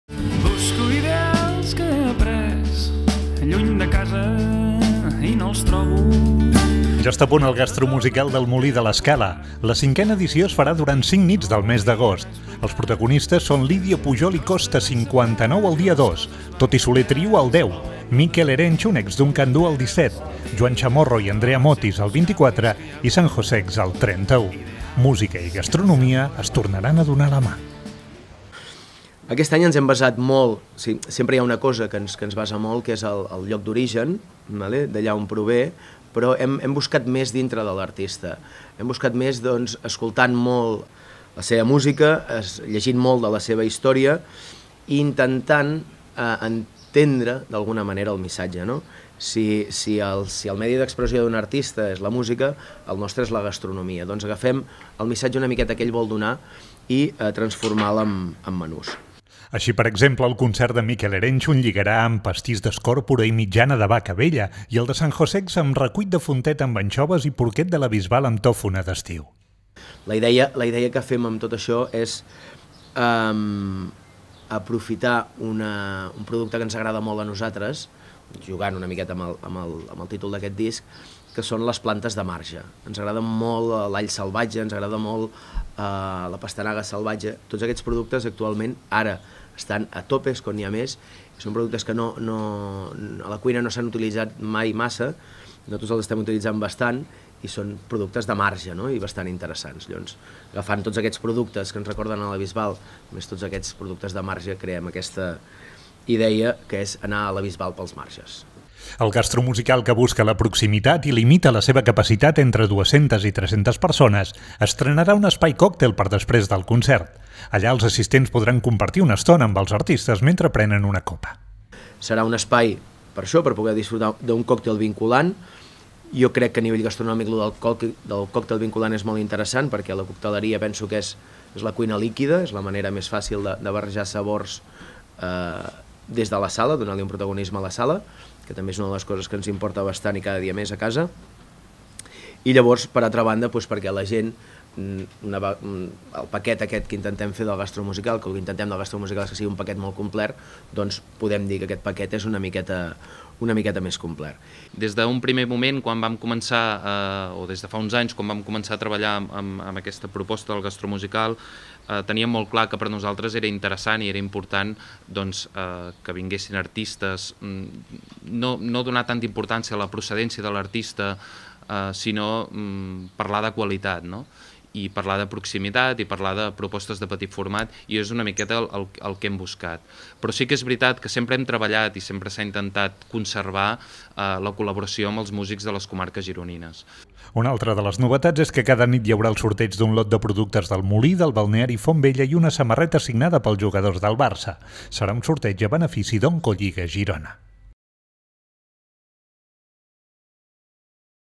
Mikel Erentxun, Sanjosex, Andrea Motis i altres al Gastromusical d'El Molí
La cinquena edició del Gastromusical del restaurant El Molí de l'Escala presenta el mes d'agost Lídia Pujol i Costa 59 (dia 2), Toti Soler (dia 10), Mikel Erentxun (dia 17), Joan Chamorro & Andrea Motis (dia 24) i Sanjosex (dia 31).